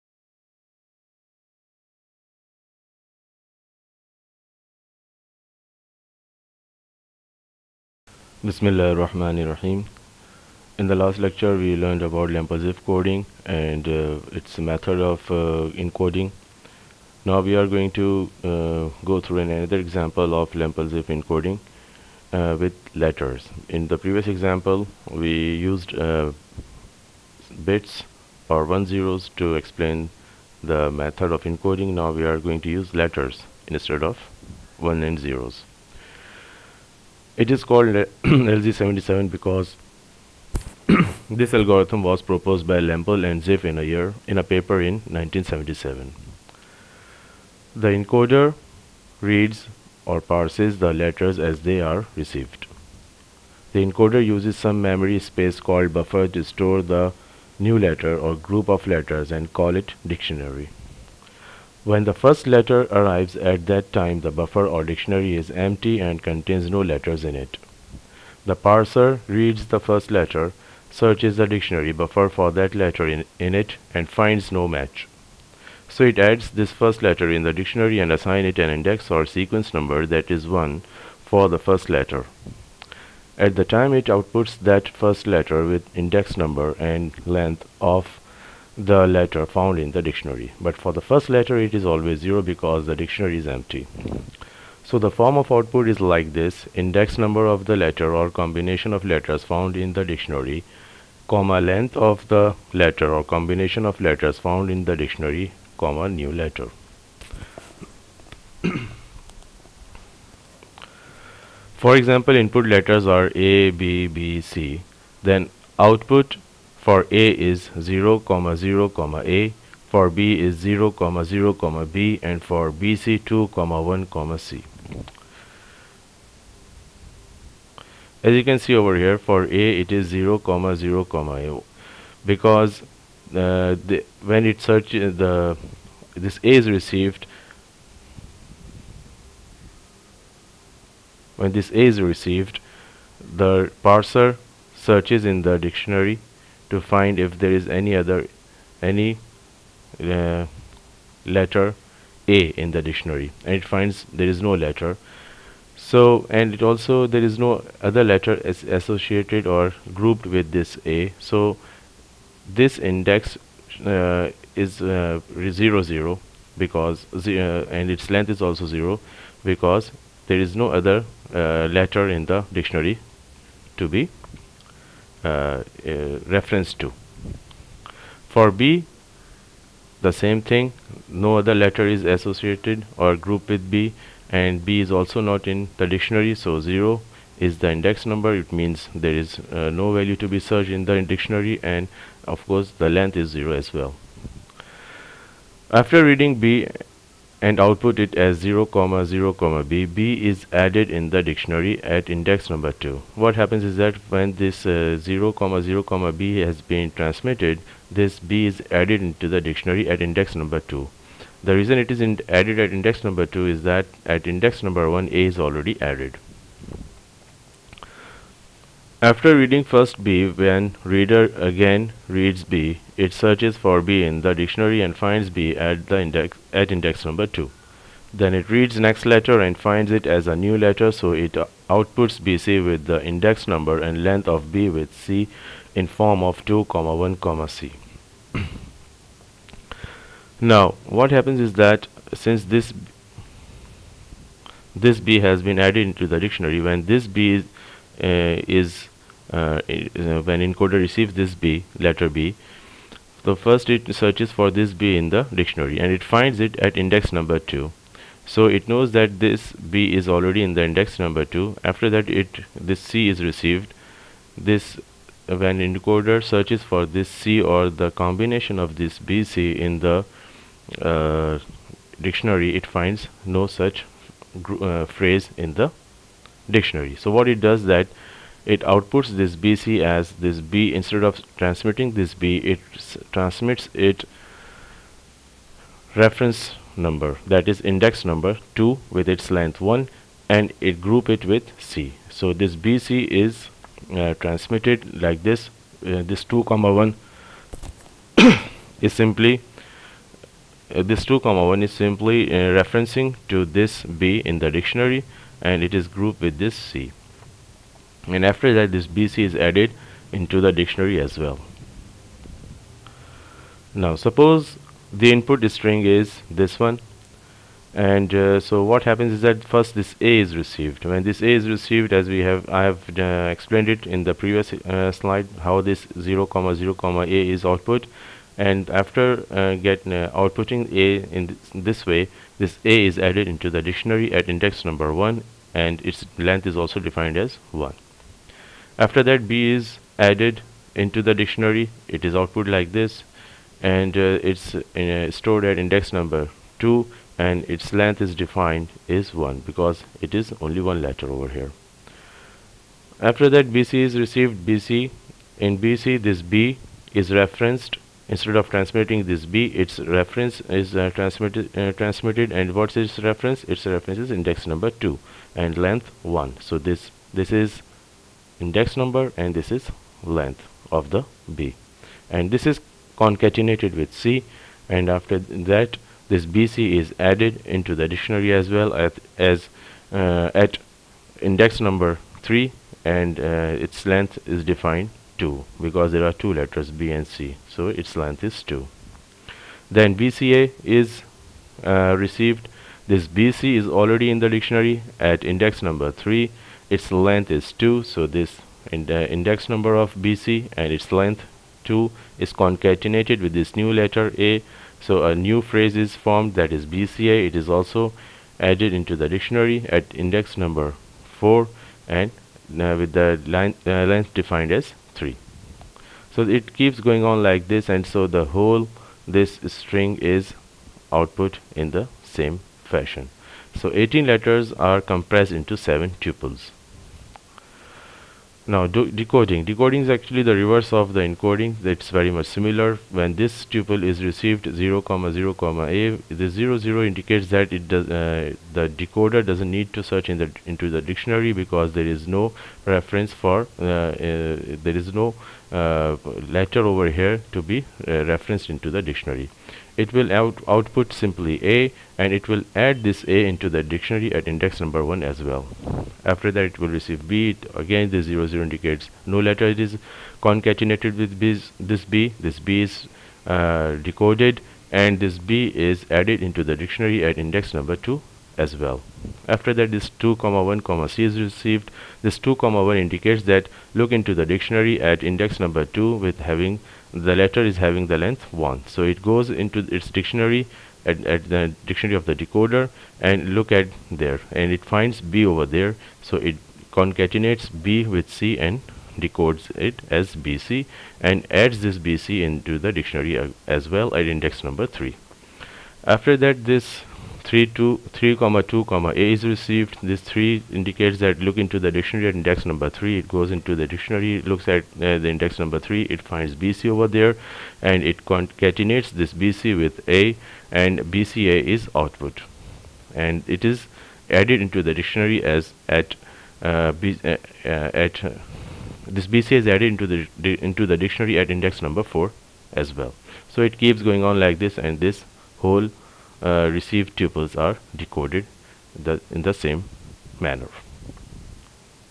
Information Coding Theory Part 6 - Lampel Ziv coding example with letters
Information Coding Theory
Topics: Source Coding - Lampel Ziv coding example with letters
By Dr. Waheeduddin Hyder